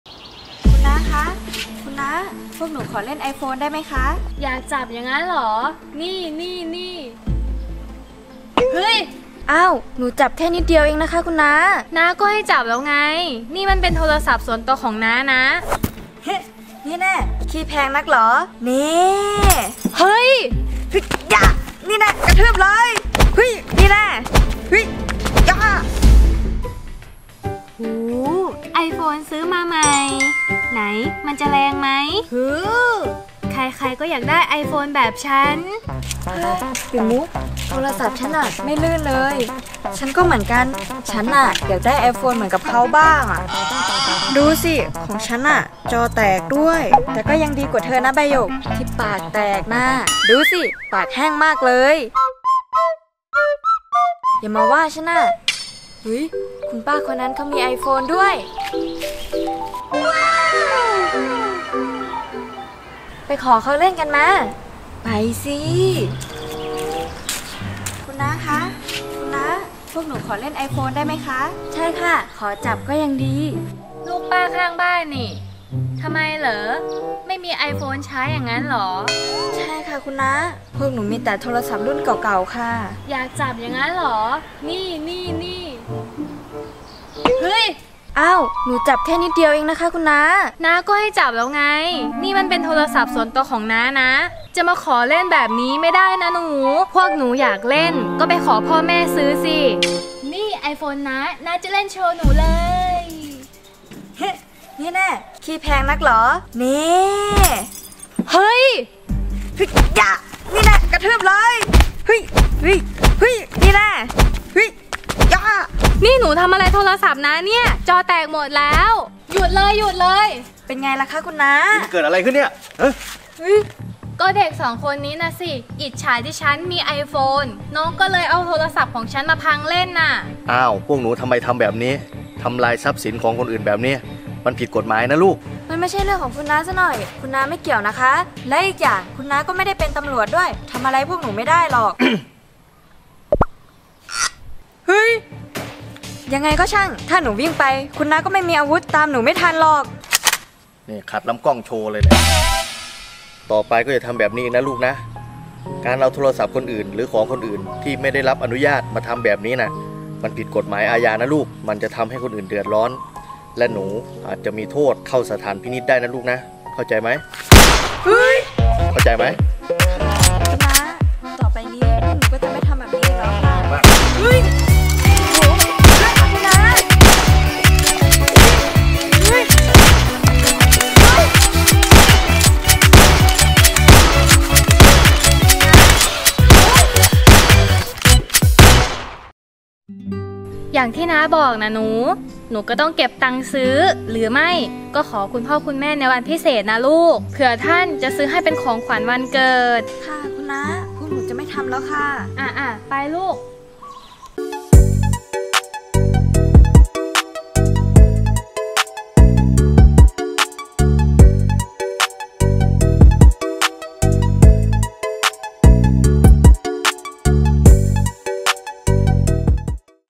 แย่ง iPhone คนอื่นมาทำลายทิ้ง (ละครสั้นสอนใจ)
ปิ่นมุกใบหยกอิจฉาที่ป้านกยูงมี iphone
เลยขอจับแต่พอเขาไม่ให้เล่นก็พากันทำลายโทรศัพท์ของป้านกยูง
อย่าพังไอโฟน

เจ้าของเดียวกับ Tongtang Family TV